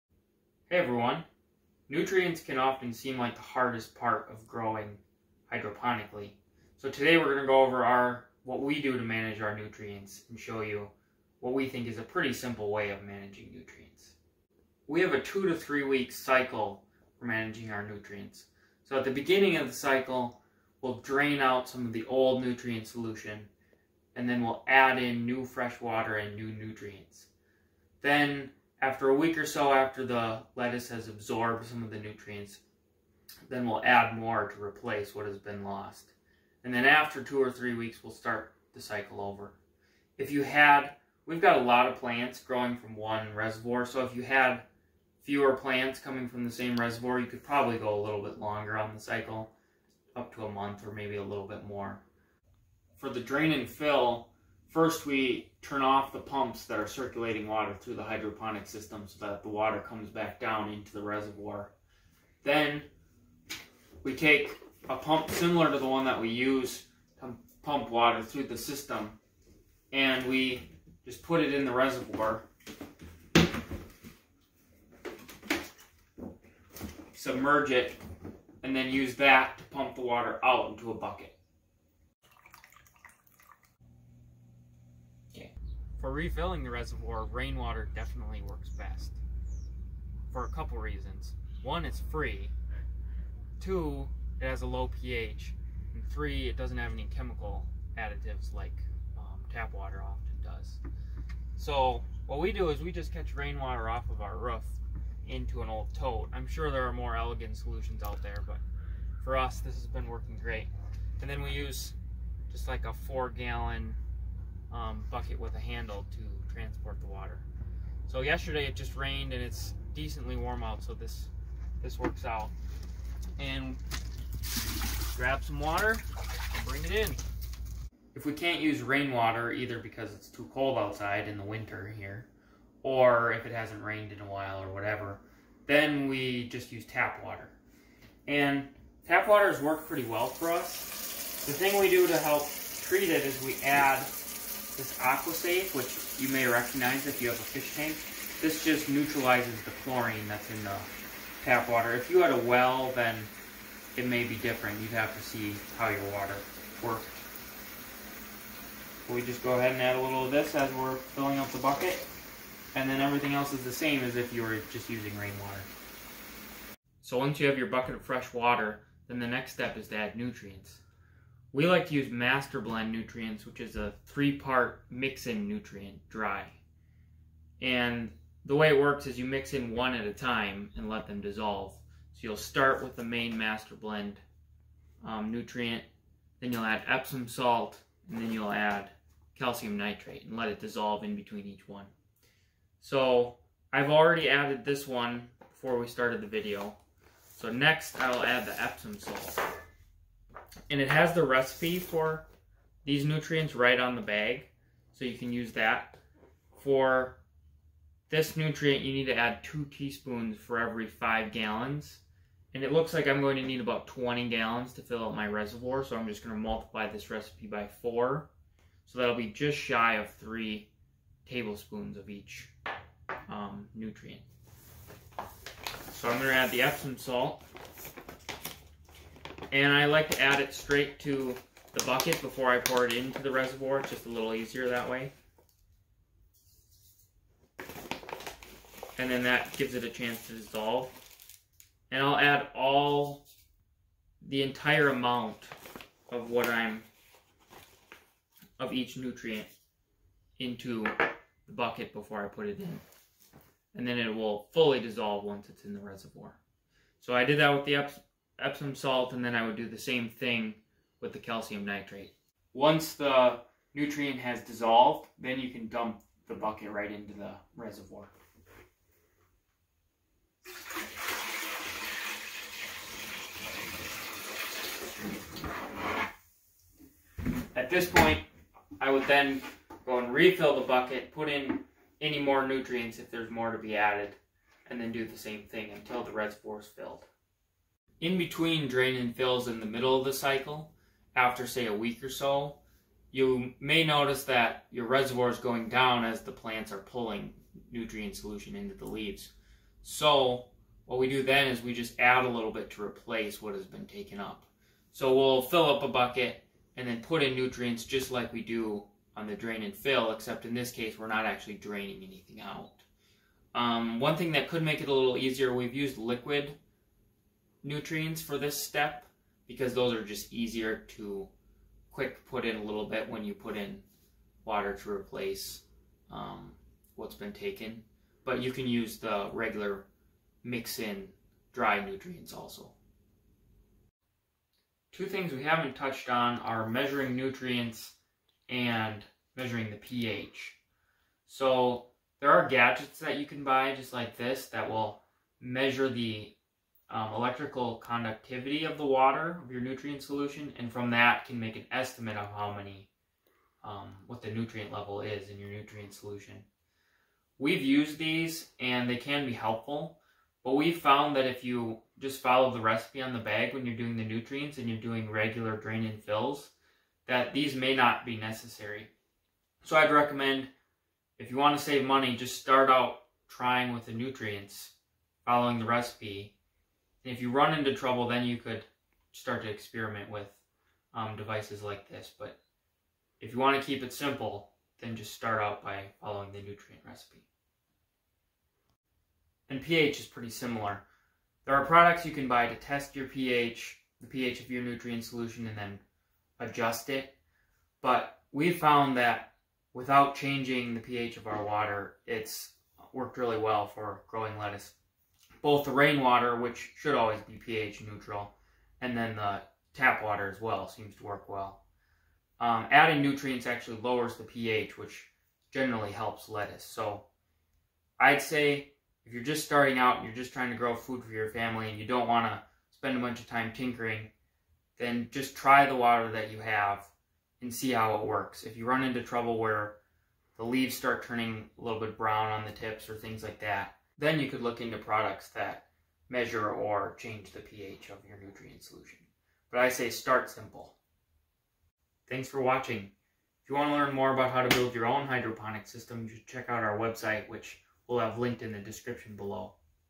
Simple Approach to Hydroponic Lettuce Nutrients
In this video we describe the simple method we use for managing the nutrient levels in our home DIY hydroponic lettuce system. Our goal is to be as simple, low maintenance and low-cost as possible, while still growing full and delicious heads of lettuce!

Here's a link to the nutrients we use (these are affiliate links so if you make a purchase using them, we may earn a small commission at no cost to you):

Master Blend (mix-in)
Dynagrow (liquid)